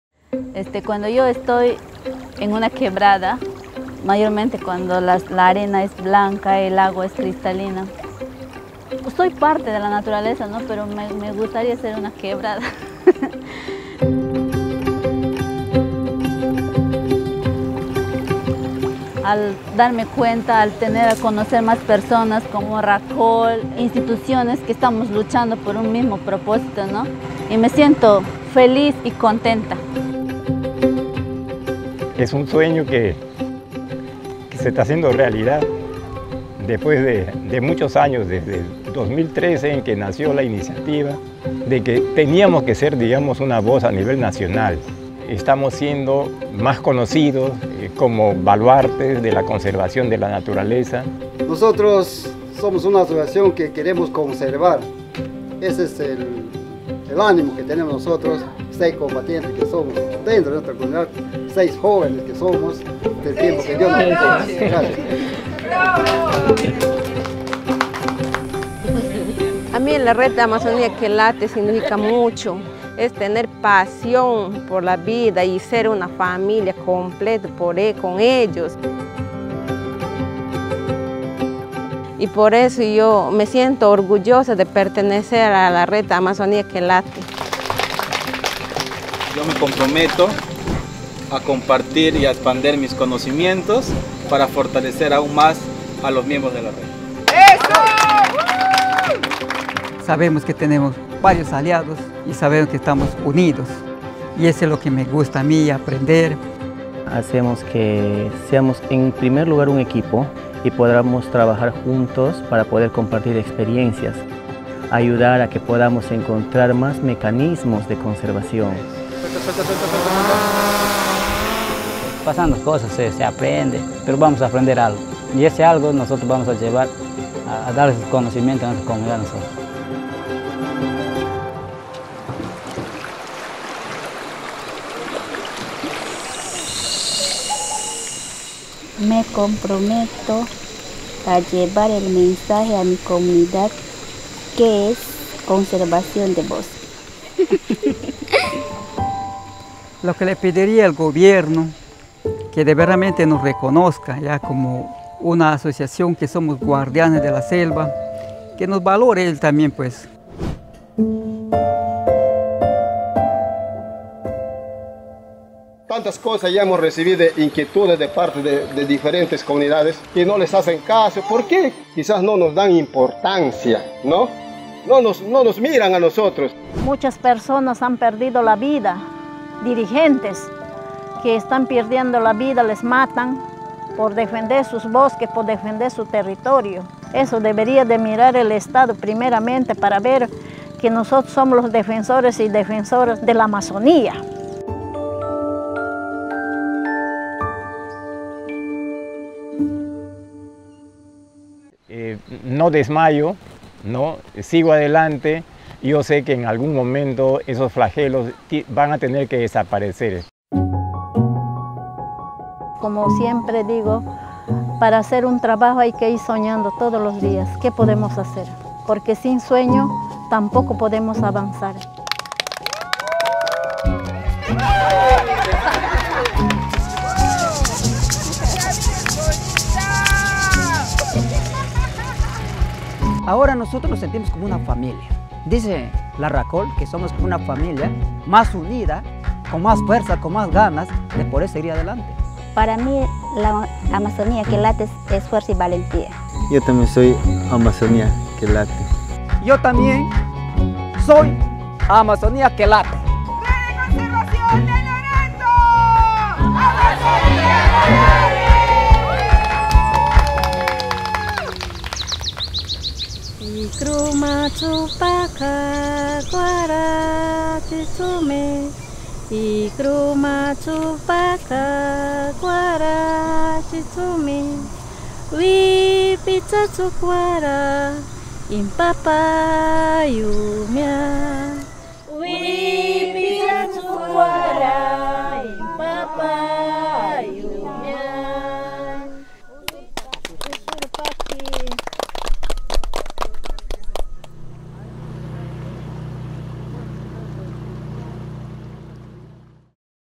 RACOL, Somos la Red Amazónica de Conservación de Loreto.
🤜🤛 Estamos felices de invitarte a revivir nuestro Encuentro Regional Amazonía que Late en Loreto, que en esta versión 2022, tuvo como escenario la Comunidad de Puerto Prado, titular del ACP Paraíso Natural de Iwirati.

⭐ La familia de RACOL y nuestros aliados hemos compartido un gran espacio de fortalecimiento. Un espacio para el intercambiado de nuestras experiencias y por supuesto, un espacio para celebrar, con la alegría que nos caracteriza, la gran labor que realizamos para la conservación de nuestra Amazonía.

🌳¡Somos una Red unida, como una gran familia, empoderada y comprometida con la VIDA!

Sé parte de nuestra experiencia ¡Todas y todos somos Amazonía que Late!